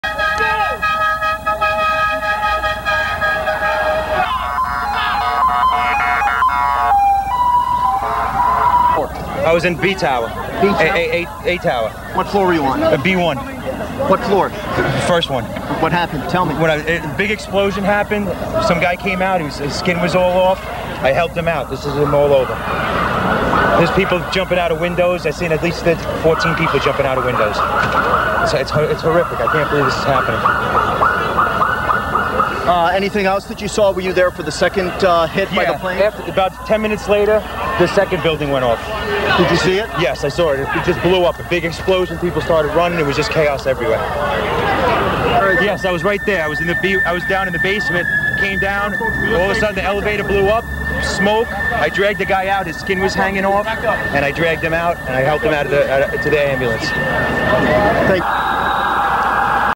Kenny Johannemann and Basement Explosions
Kenny Johannemann worked as a part-time janitor in the World Trade Center when it was attacked and destroyed on September 11, 2001.  He was in the North Tower's basement waiting for an elevator when the first explosion occurred.